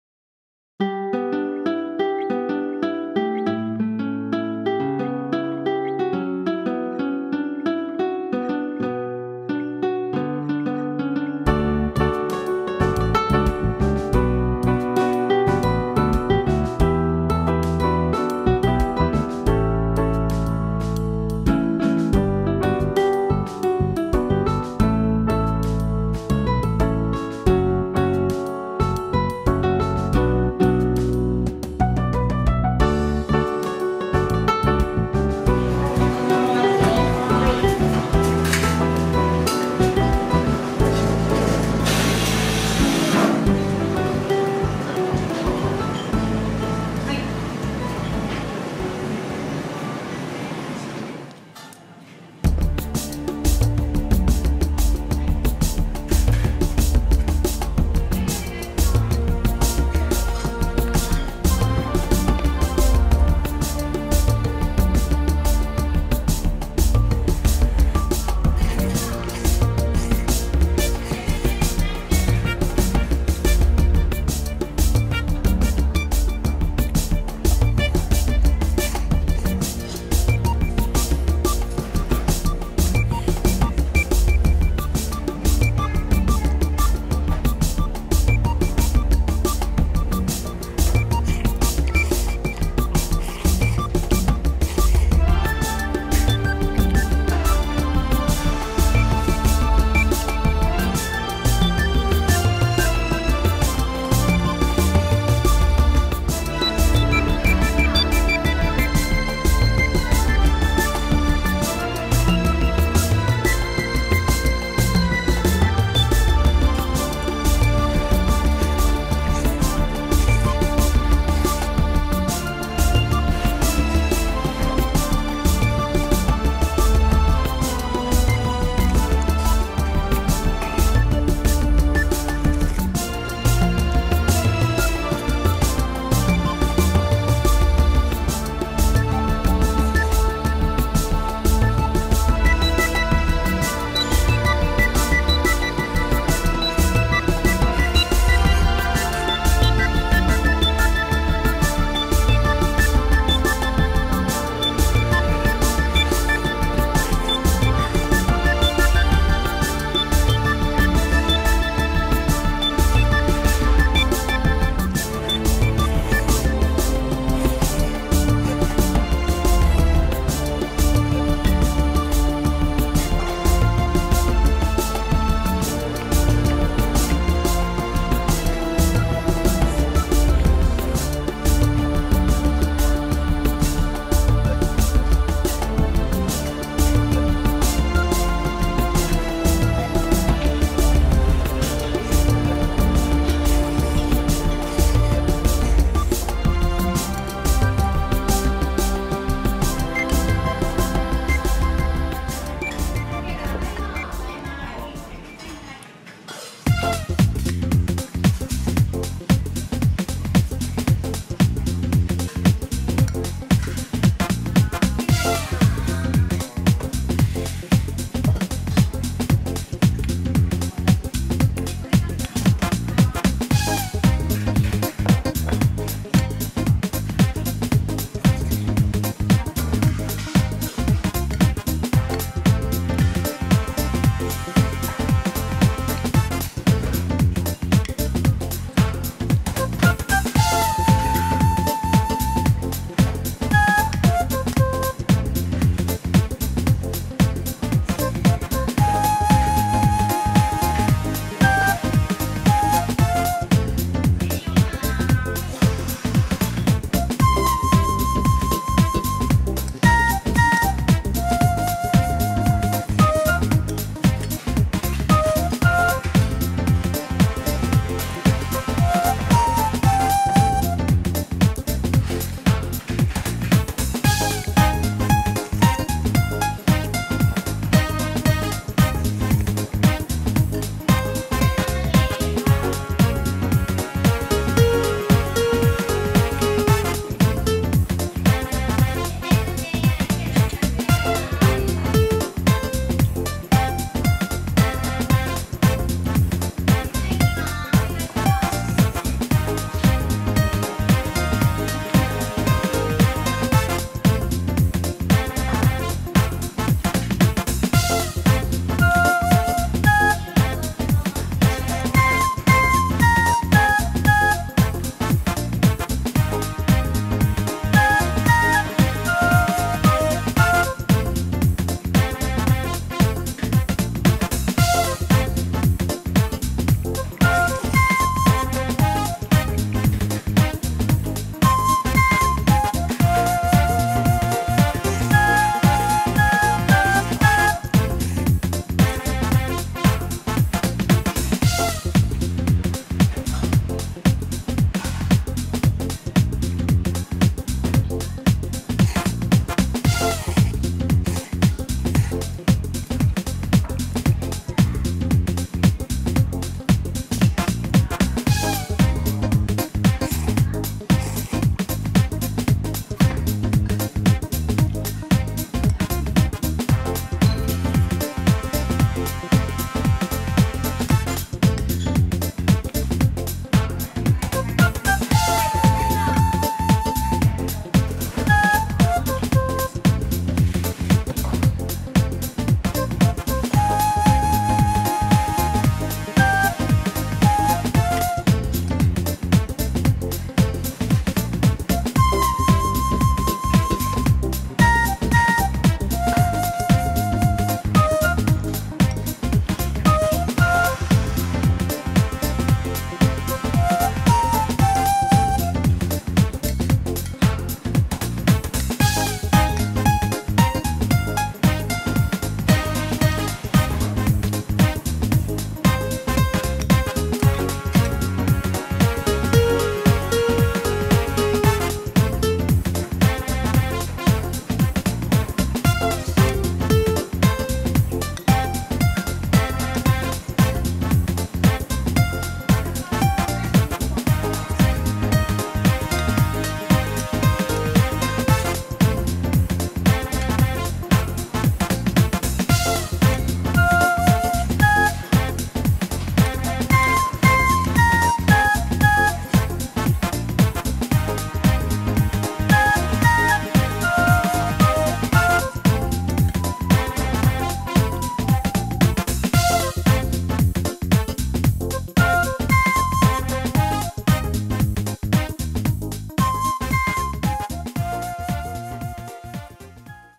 【ウエスト】③ 懐かしいゴボ天うどんと金曜日カレーを食す ＃584
ウエスト千葉旭町店で博多うどんとごぼう天と金曜日カレーをいただきました。　　A ❤️❤️❤️❤️   登録お願い致します　❤️❤️❤️H